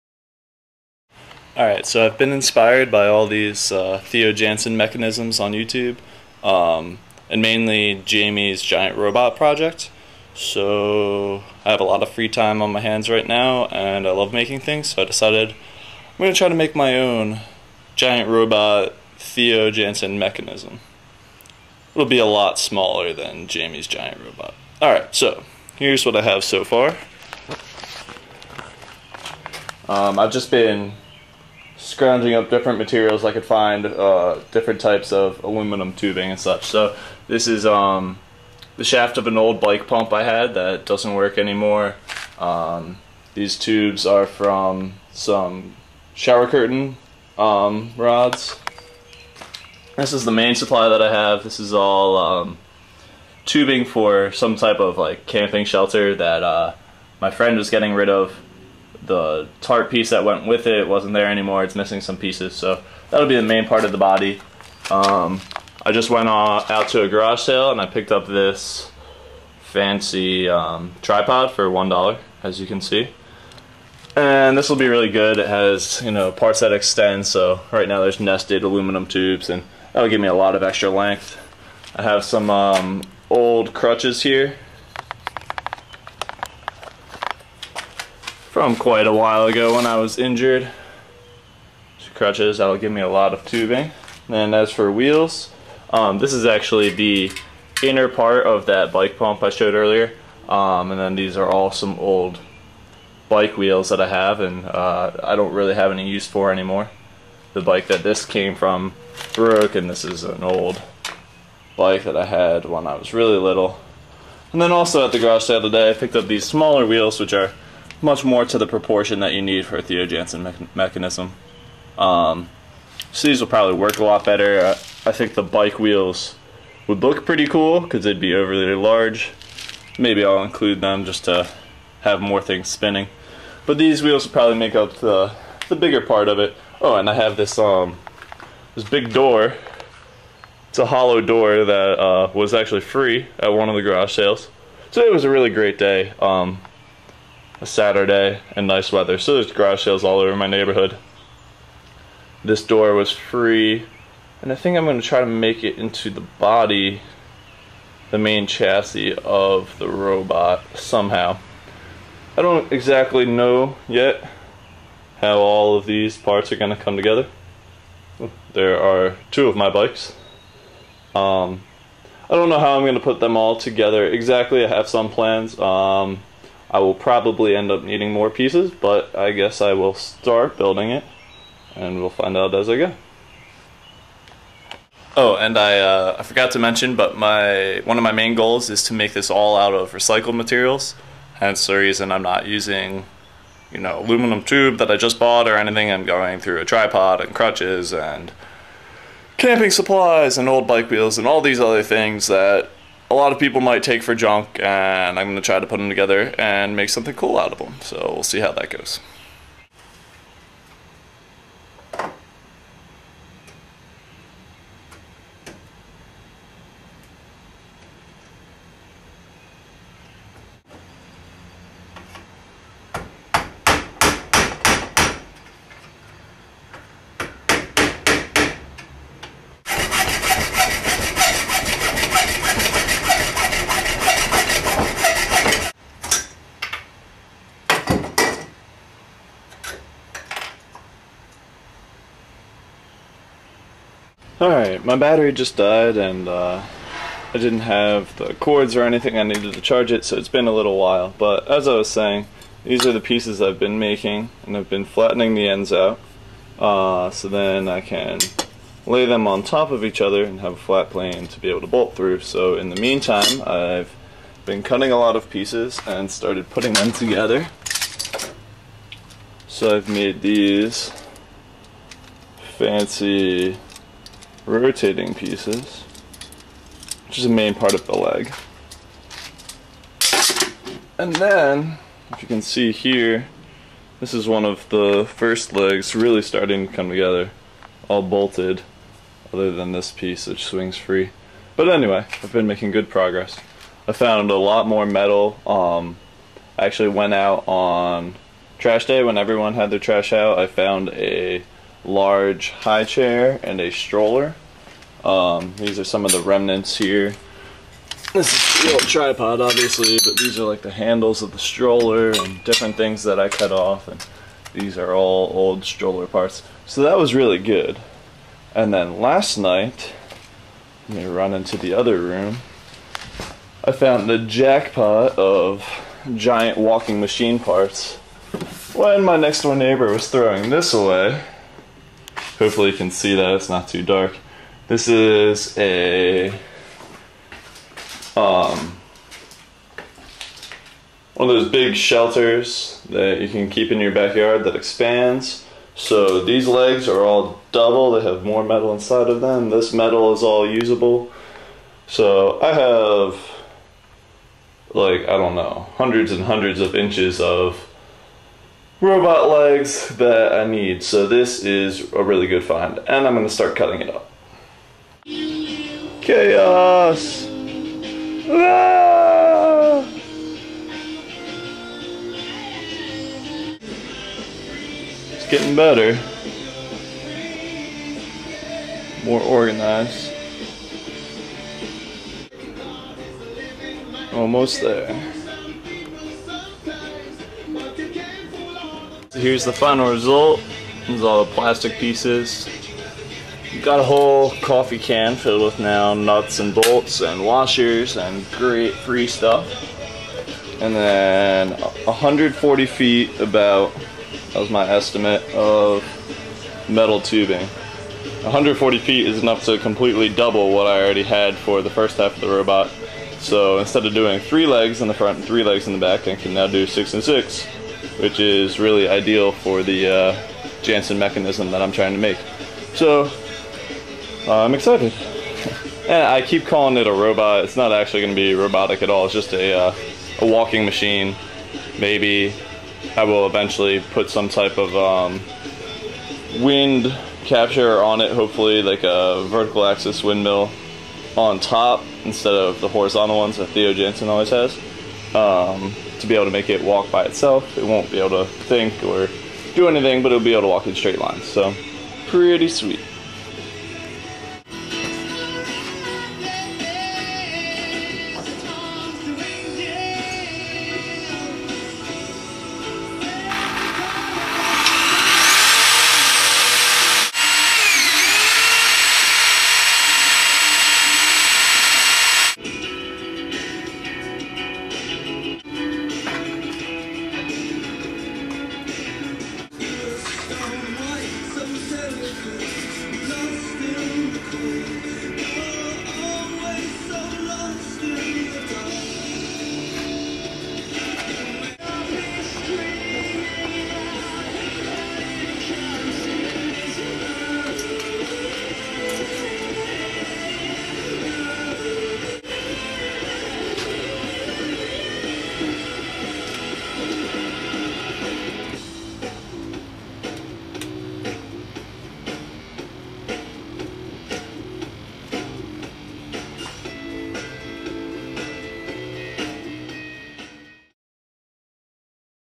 Theo Jansen Mechanism 1 - Getting Started
Here is my first video of the construction of my Theo Jansen Mechanism.  I tried to create this machine using as many recycled materials as possible (I only spent $2 on materials besides nuts bolts and washers).  I did not invent the mechanism of course, but the particular design of this machine is my own invention, for better or for worse.

I continually mention Jamie's Giant Robot Project.  Check out his videos if you are not already a fan